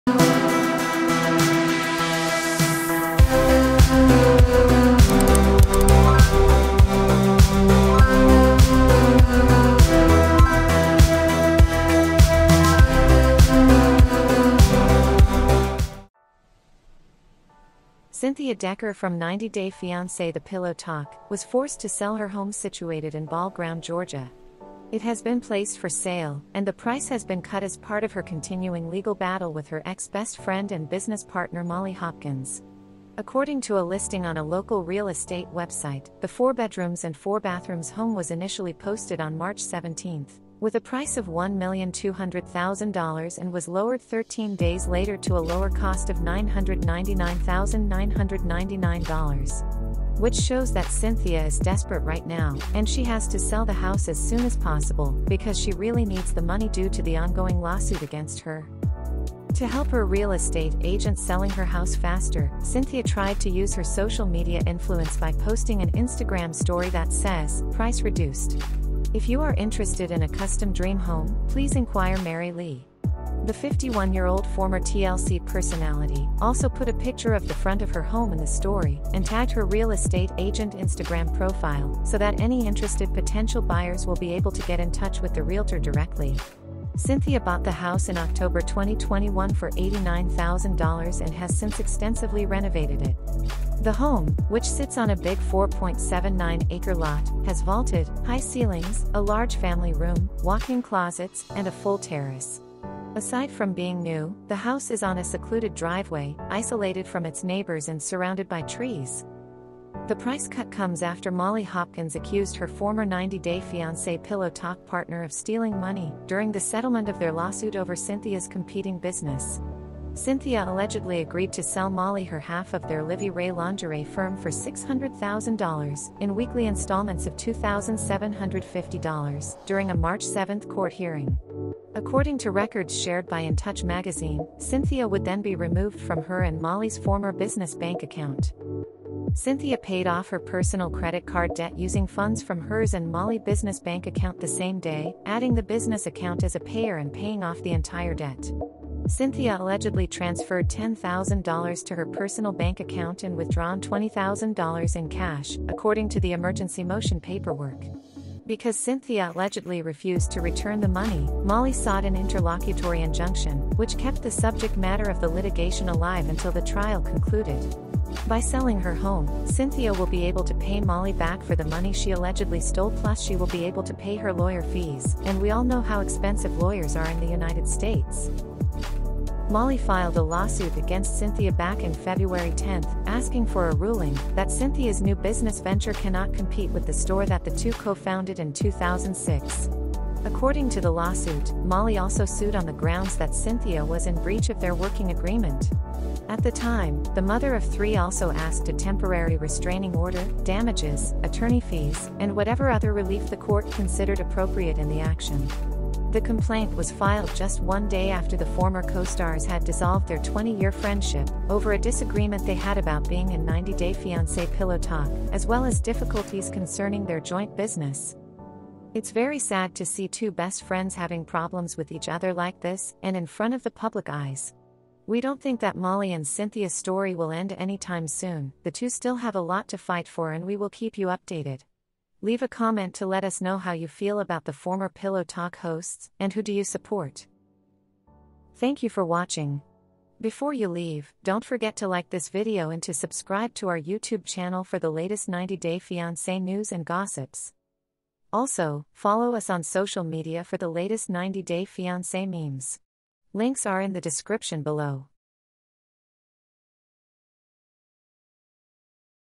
90 day fiance: Cynthia Decker had to sell her house while being sued by Molly Hopkins
90 day fiancé: Pillow talk reality star Cynthia Decker was forced to sell her house situated in Ball Ground, Georgia after being sued by Molly Hopkin, them lawyers are so expensive!